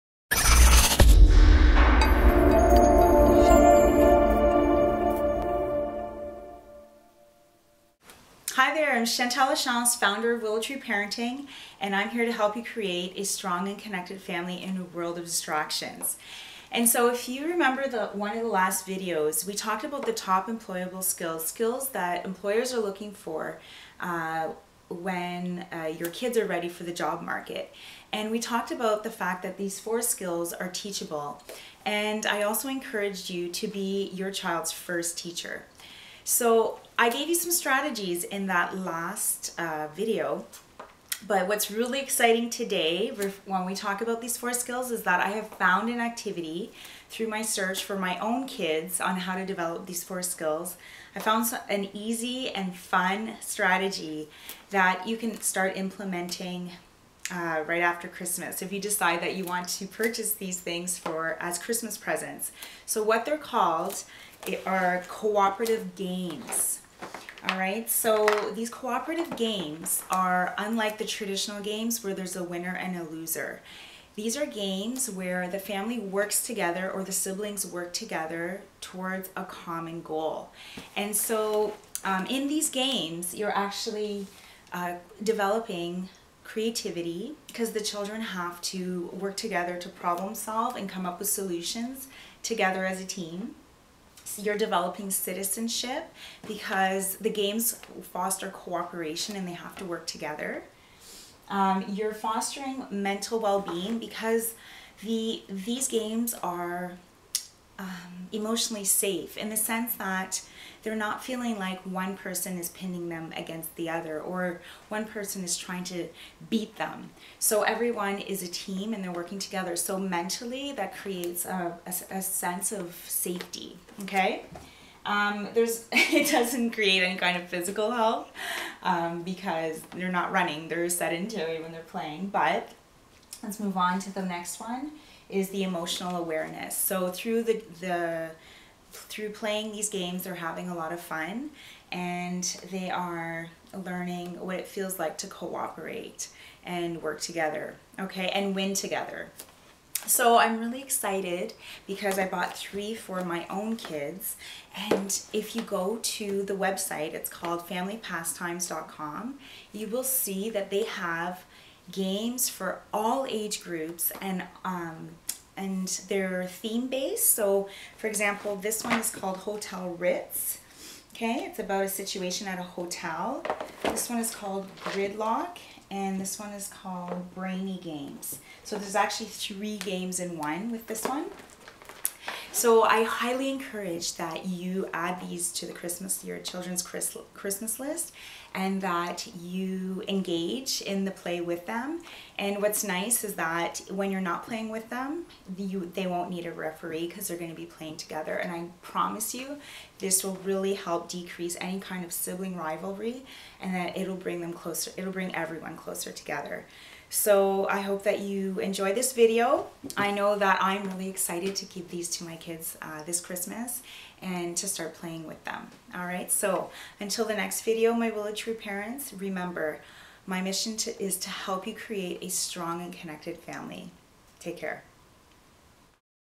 How Do I End Sibling Rivalry?/Getting Kids To Get Along
Here is the best idea yet in order to end sibling rivalry and foster cooperation in the home!  Le't have fun and put these kids back on the same team! Fun and easy!!!

Transcript:
Hi, I’m Chantale Lachance, founder of Willow
tree parenting and I'm here to help you
create a strong and connected family in
the world of distractions and so if you
remember the one of the last videos we
talked about the top employable skills
skills that employers are looking for
when your kids are ready for the job
market and we talked about the fact that
these four skills are teachable and I
also encouraged you to be your child's
first teacher so I gave you some
strategies in that last video but what's
really exciting today when we talk about
these four skills is that I have found
an activity through my search for my own
kids on how to develop these four skills
I found an easy and fun strategy that
you can start implementing right after
Christmas if you decide that you want to
purchase these things for as Christmas
presents so what they're called are
cooperative games all right so these
cooperative games are unlike the
traditional games where there's a winner
and a loser these are games where the
family works together or the siblings
work together towards a common goal
and so in these games you're actually
developing creativity because the
children have to work together to
problem-solve and come up with solutions
together as a team you're developing
citizenship because the games will
foster cooperation and they have to work
together you're fostering mental
well-being because the these games are
emotionally safe in the sense that
they're not feeling like one person is
pinning them against the other or one
person is trying to beat them so
everyone is a team and they're working
together so mentally that creates a
a sense of safety okay
there's it doesn't create an kind of
physical health because they're not
running there is that enjoy when they're
playing but let's move on to the next
one is the emotional awareness so
through the through playing these games
are having a lot of fun and they are
learning what it feels like to cooperate
and work together okay and win together
so I'm really excited because I bought
three for my own kids and if you go to
the website it's called family pastimes
calm you will see that they have games
for all age groups and um and their
theme based so for example this one is
called Hotel Ritz okay it's about a
situation at a hotel this one is called
gridlock and this one is called brainy
games so this is actually three games in
one with this one so I highly encourage
that you add these to the Christmas your
children's Christmas Christmas list and
that you engage in the play with them
and what's nice is that when you're not
playing with them you they won't need a
referee because they're going to be
playing together and I promise you this
will really help decrease any kind of
sibling rivalry and that it'll bring
them closer it'll bring everyone closer
together so I hope that you enjoy this
video I know that I'm really excited to
keep these to my kids this Christmas and
to start playing with them
alright so until the next video my
willow tree parents remember my mission
to is to help you create a strong and
connected family.